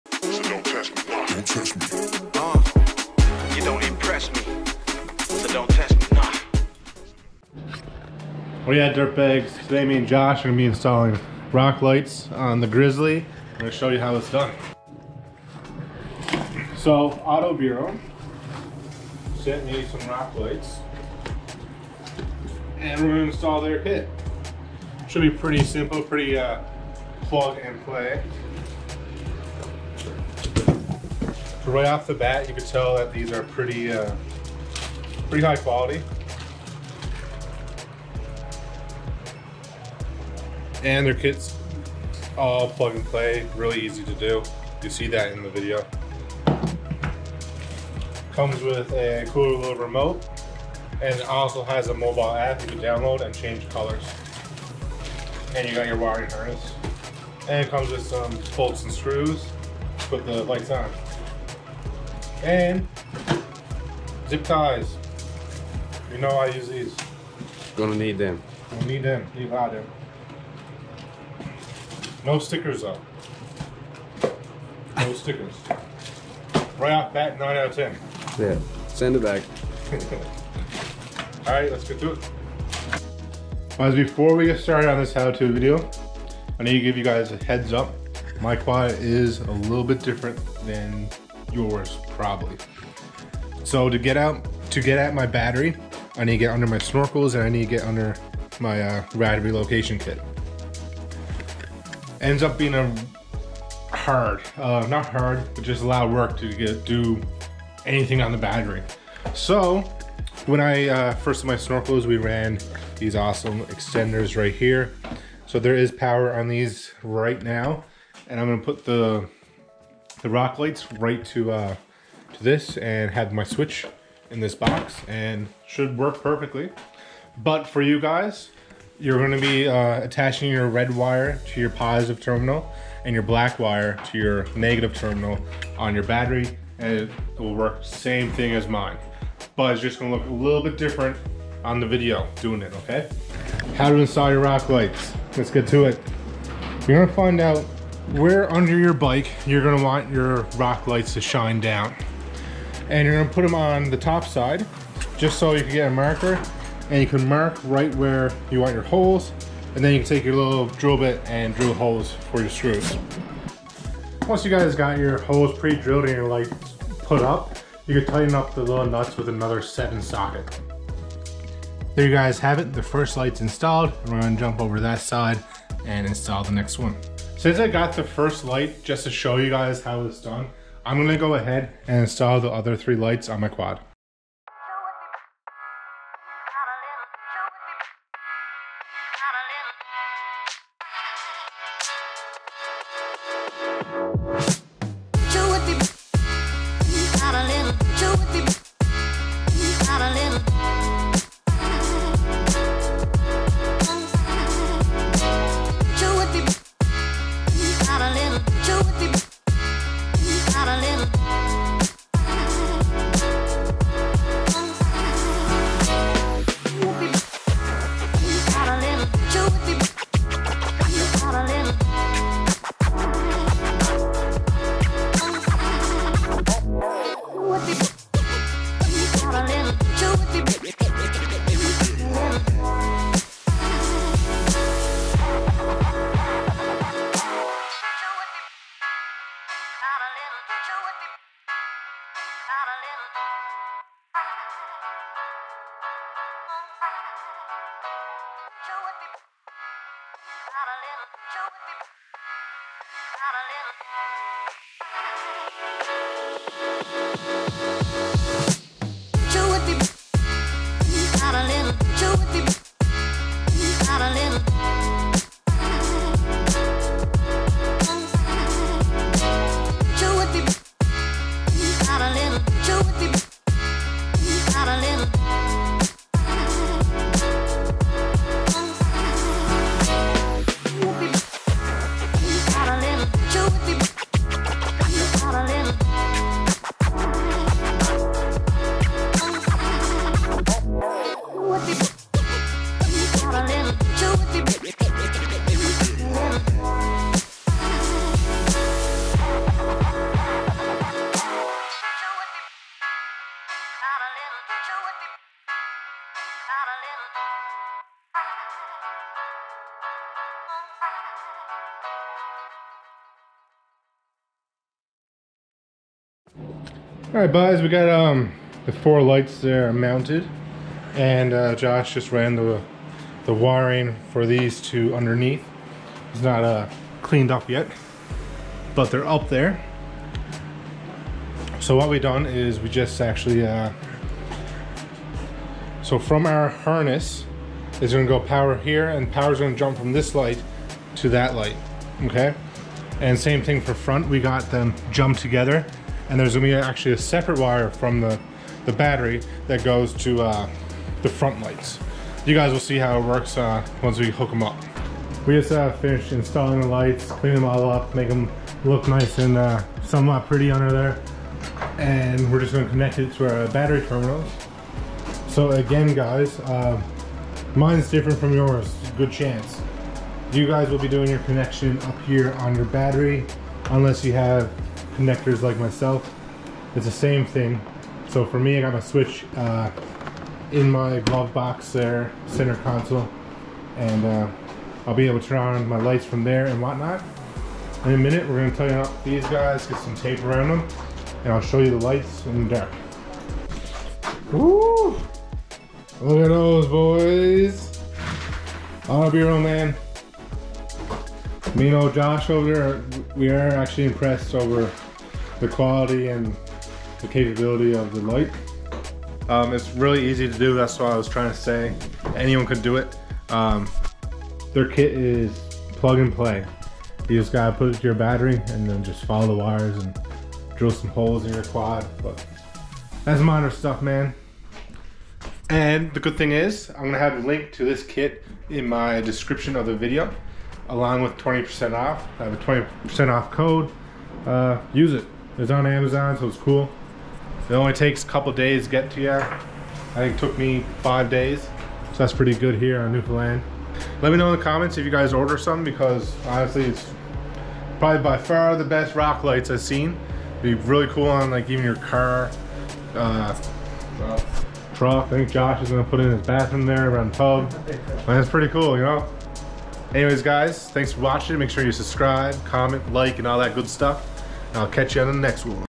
Easy Rock Light Installation for Your ATV
Auto Buero makes it really easy for the install, connect the harnes to the batter and from there its all plug and play!

SAVE WITH THE CODE:

offroaddan20